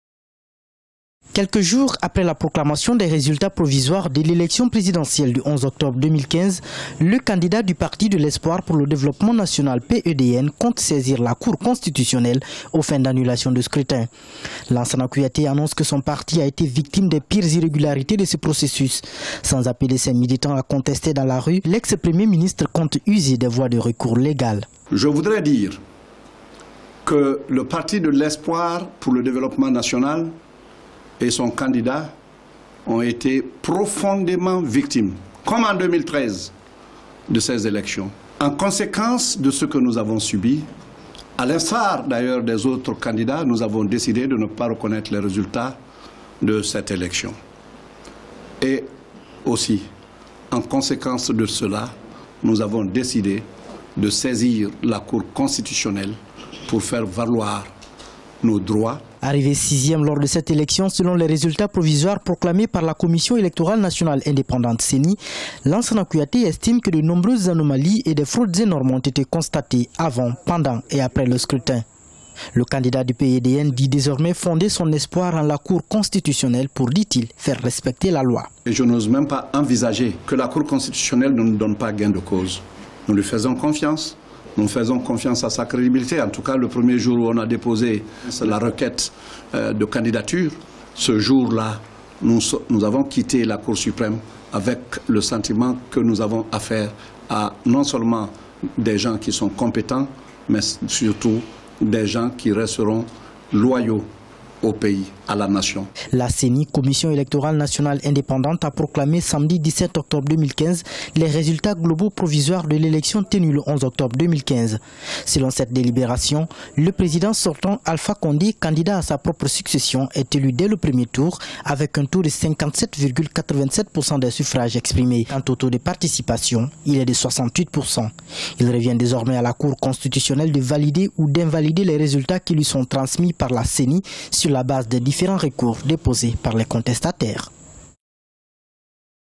Guinée, Une procédure aux fins d'annulation du scrutin
Politique, GUINEE: Alors que certains candidats ont décidé de se retirer du processus électoral en Guinée, d'autres vont entamé des procédures en appel au près de la cour constitutionnelle.